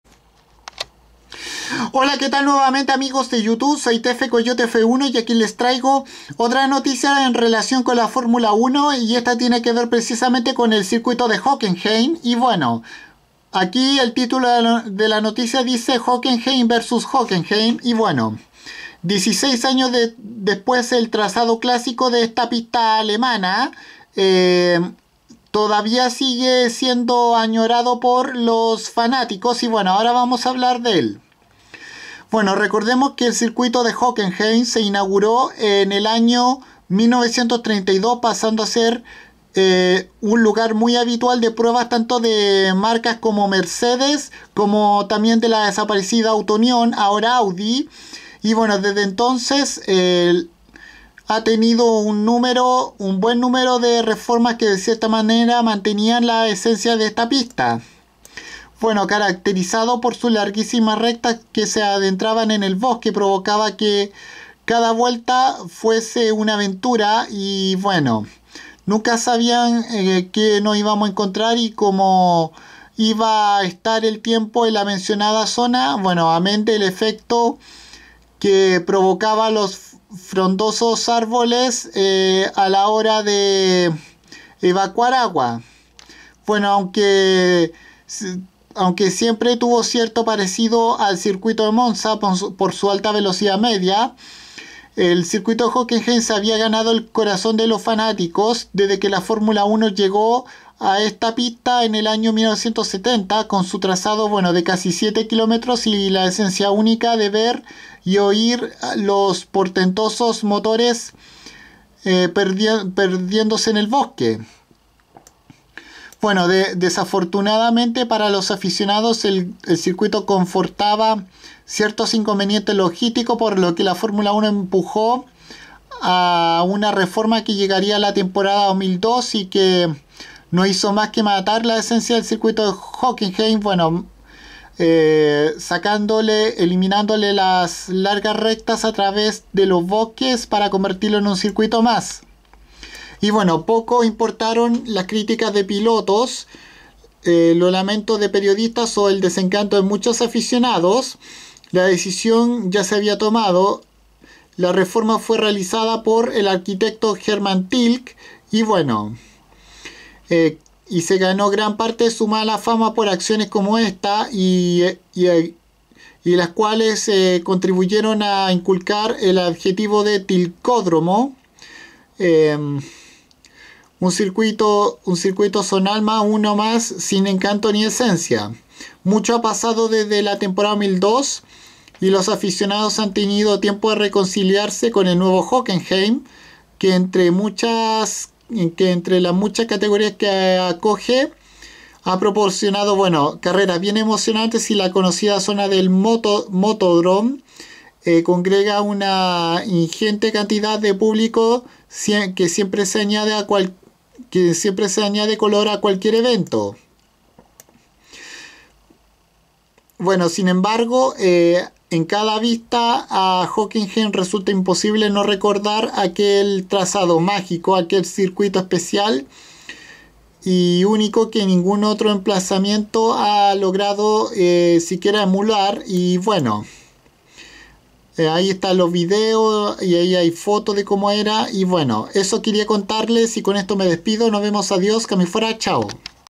Hockenheim vs. Hockenheim
En este vídeo les hablo sobre como fue antes y es ahora el Hockenheimring la pista en la que se disputará el GP de Alemania de este fin de semana en la Temporada 2018 de la F1